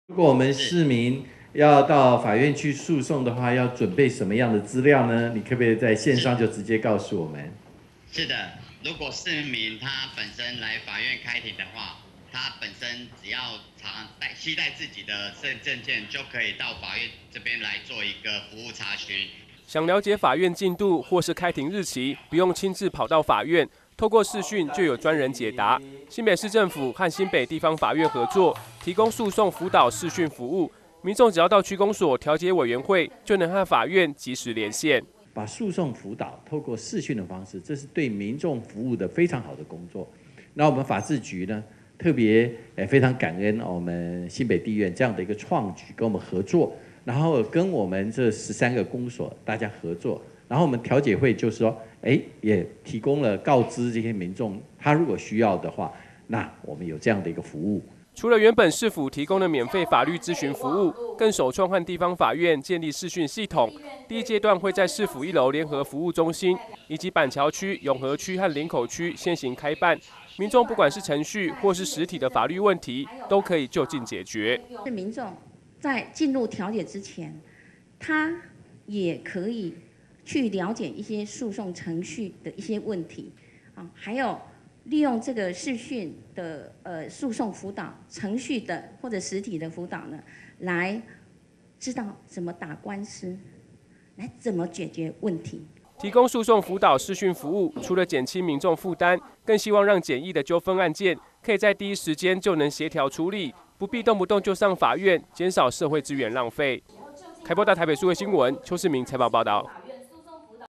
20170705 糾紛訴訟免上法院 新北啟動視訊諮詢服務
糾紛訴訟免上法院 新北啟動視訊諮詢服務 (凱擘大台北數位新聞)

遇到法律訴訟問題，很多人都想到要求助律師，為了幫民眾減輕負擔，新北市政府除了提供免費的法律諮詢服務，現在更和新北地方法院合作，首創訴訟輔導視訊服務，民眾只要到區公所調解委員會，就可以和法院即時連線，由專人線上解決民眾的問題。

想了解法院案件進度或是開庭日期，不用親自跑到法院，透過視訊就有專人解答。新北市政府和新北地方法院合作，提供訴訟輔導視訊服務，民眾只要到區公所調解委員會，就能和法院即時連線。

除了原本市府提供的免費法律諮詢服務，更首創和地方法院建立視訊系統，第一階段會在市府1樓聯合服務中心，以及板橋區、永和區和林口區先行開辦，民眾不管是程序或是實體的法律問題，都可以就近解決。

提供訴訟輔導視訊服務，除了減輕民眾負擔，更希望讓簡易的糾紛案件，可以在第一時間就能協調處理，不必動不動就上法院，減少社會資源浪費。

邱世明 採訪報導 (新北市/法律諮詢)